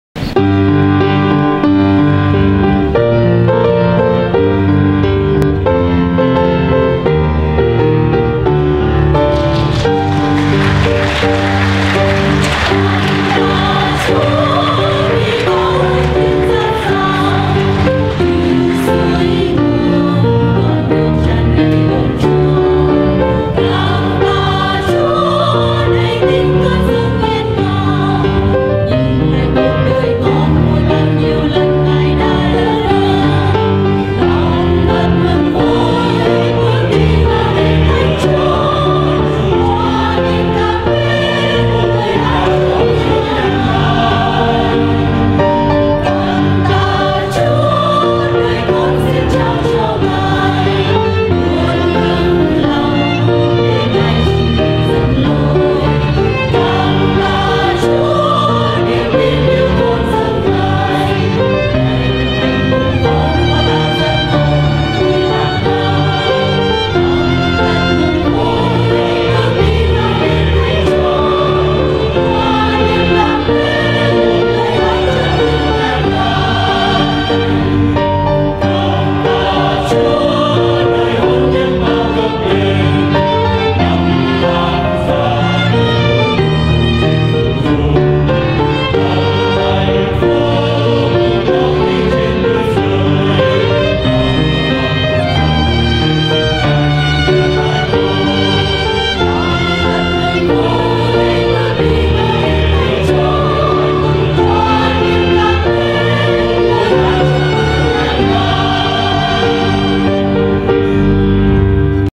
Cảm Tạ Chúa__April 10th 2015
Cảm Tạ Chúa
60 Năm Hồng Ân 1955, April 10th, 2015
Ông Phêrô Trần Văn Thịnh và Bà Teresa Vũ Thị Khiết
Nhà Thờ Thánh Linh Holy Spirit Catholic Church
Fountain Valley, CA